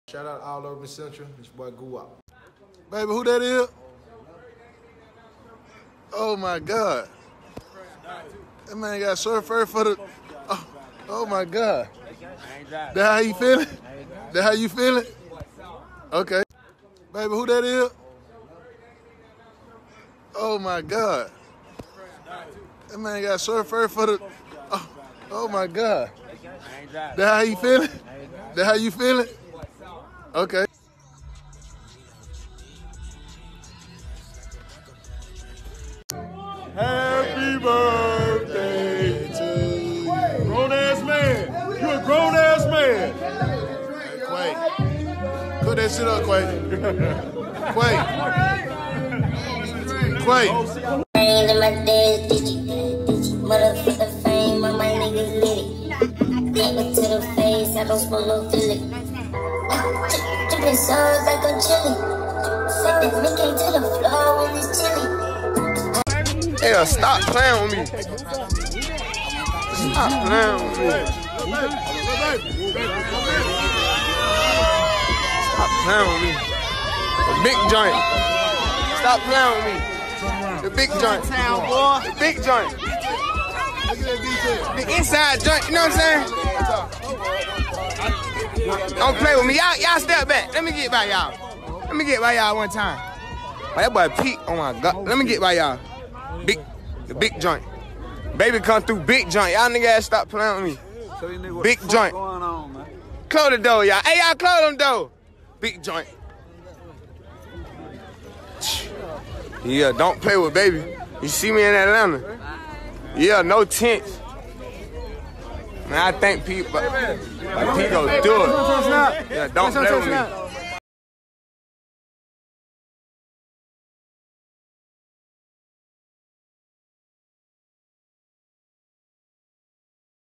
Lil Baby Pulls Up With Gucci Mane and Secret Service Valet For Rolls Royce Truck
Lil Baby Pulls Up With Gucci Mane